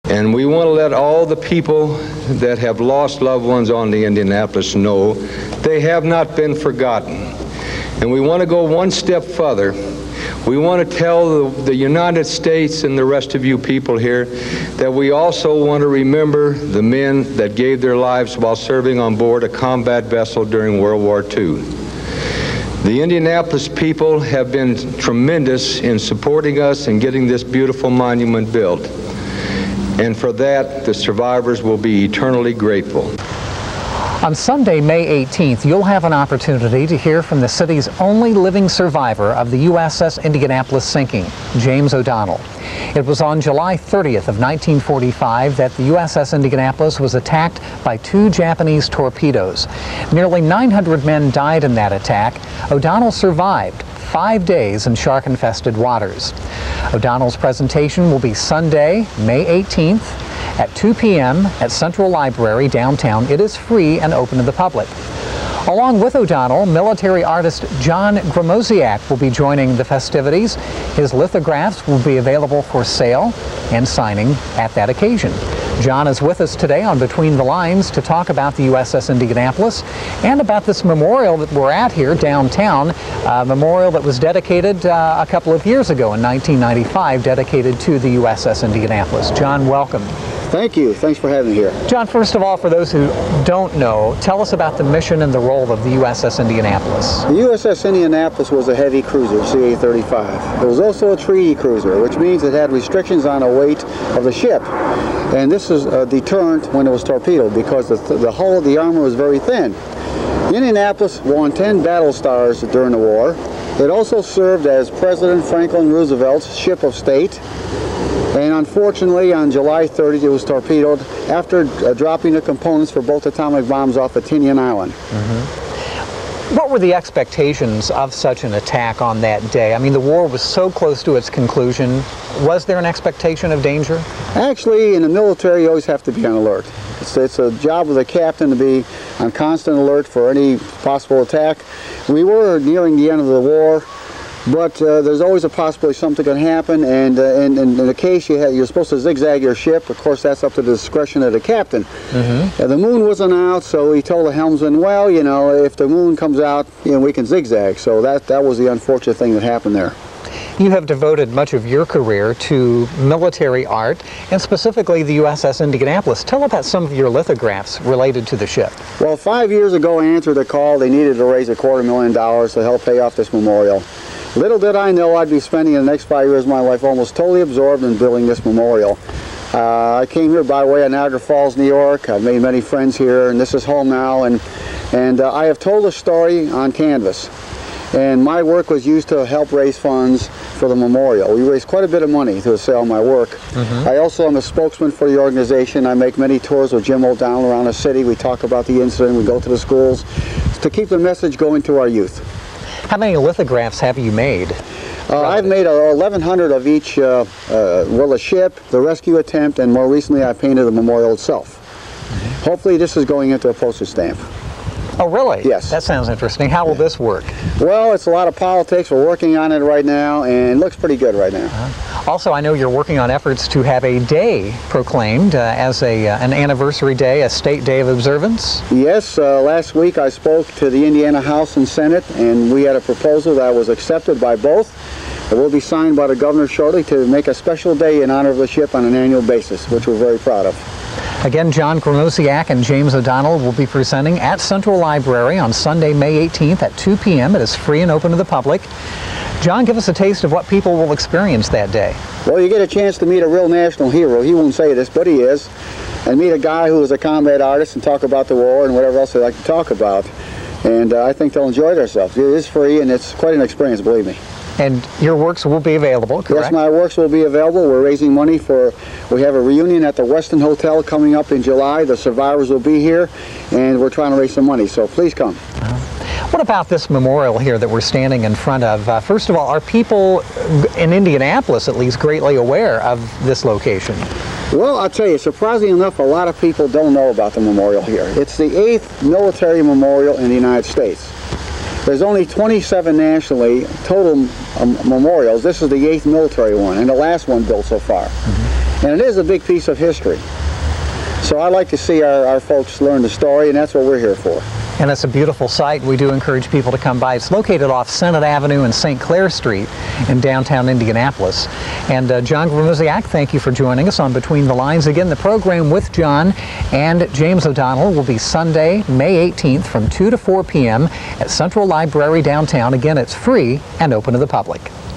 Military Artist John Gromosiak Discusses USS Indianapolis Tragedy
In this interview from May, 1997 military artist and historian John Gromosiak speaks with Jon Barnes of The Indianapolis Public Library about the sinking of the USS Indianapolis during World War II.  Gromosiak who died in December, 2011 was appearing in a Library program with the city of Indianapolis' sole living survivor of the tragedy, James E. O'Donnell, who died on January 9, 2013.  This interview took place at the USS Indianapolis Memorial on the Downtown Canal Walk which was dedicated on August 2, 1995.